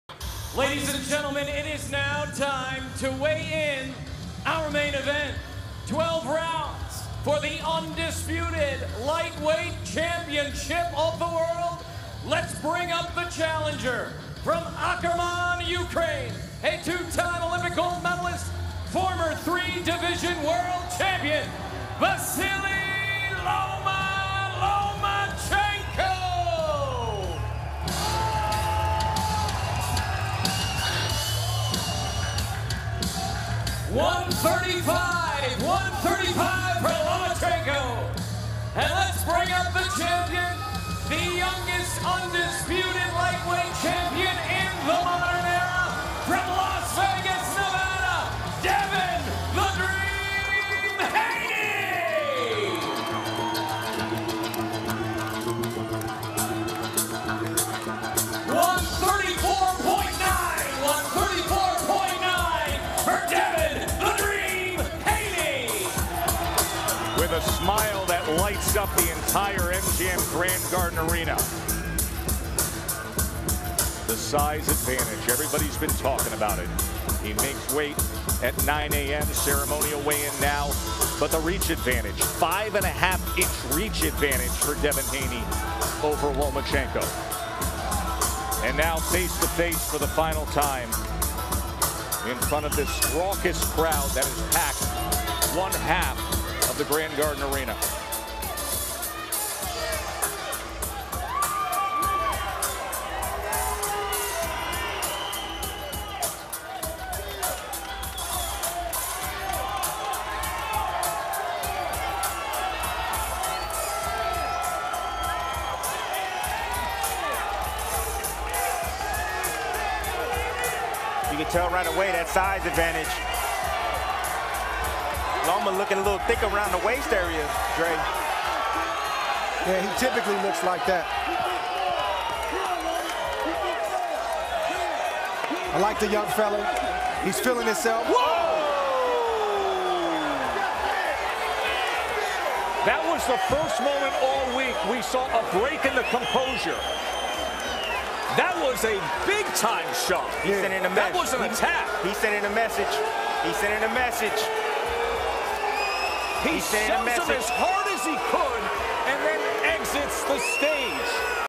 Devin Haney shoves Vasiliy Lomachenko at weigh-in - HEATED FACE OFF
Devin Haney vs Vasiliy Lomachenko

Devin Haney shoves Vasiliy Lomachenko at weigh-in; NSAC filing complaint.
Things get heated between Haney and Lomachenko during their weigh-in ahead of the Undisputed World Lightweight Title fight.